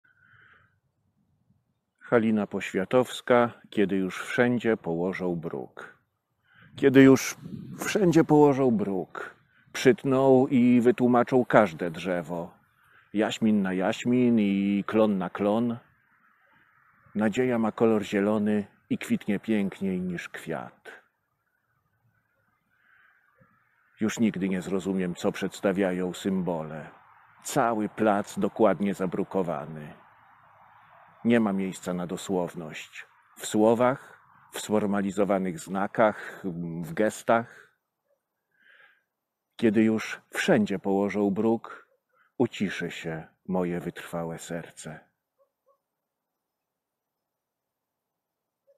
Halina Poświatowska. Kiedy już wszędzie położą bruk.
Recytacja wiersza Haliny Poświatowskiej, który właściwie to nie ma tytułu. Wszystko się zgadza. Skoro jest zarośnięty chłop zamiast delikatnej i kruchej poetki, to niech będzie też las i jezioro zamiast miasta i brukowanego placu. Co poetka miała na myśli, to ja nie wiem. Myślę, że już wtedy utrapieniem była plaga betonozy w miastach, ale nie wiem, czy to dla niej źle, czy dobrze. Trudno. Ważne, że piękne zimowe słońce przez moment złapałem, więc proszę się ponapawać. Wszystkiego dobrego w nadchodzącym niebłaganie kolejnym roku III wojny światowej. Żeby nas dalej omijała.